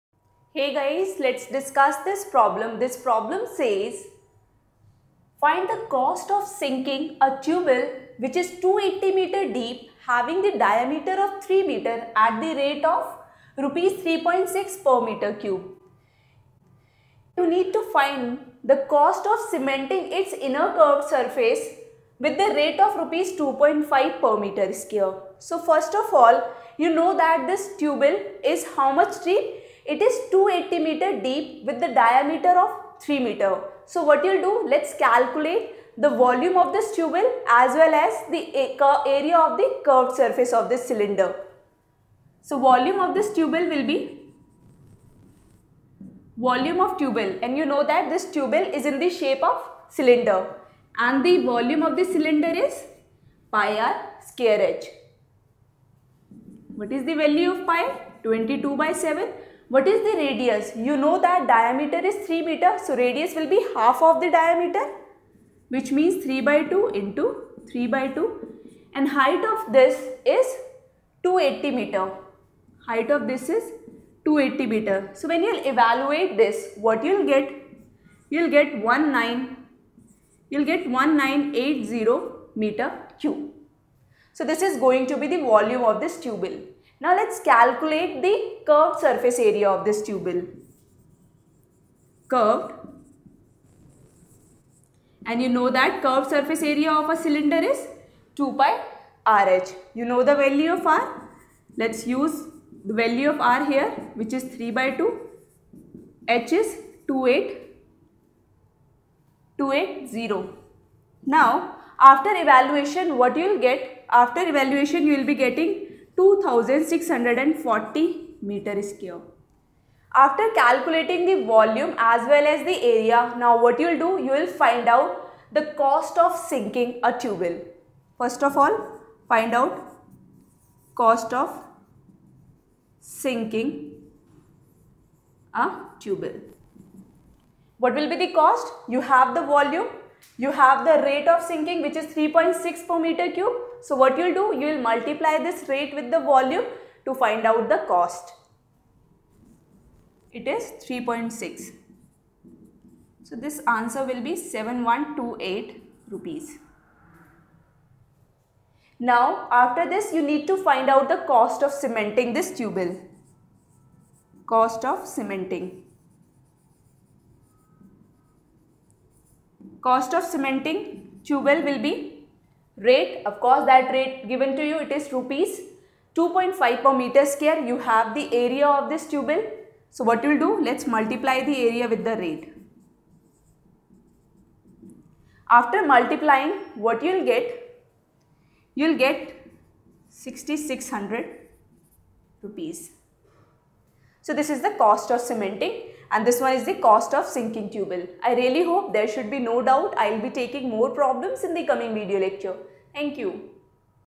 Class10th – RD Sharma – Problem-13 on Surface area and Volume of cylinder | Tutorials Point
RD Sharma - Problem 13 on Surface area & Volume of cylinder
Lecture By: Ms. Megha Agarwal, Tutorials Point India Private Limited.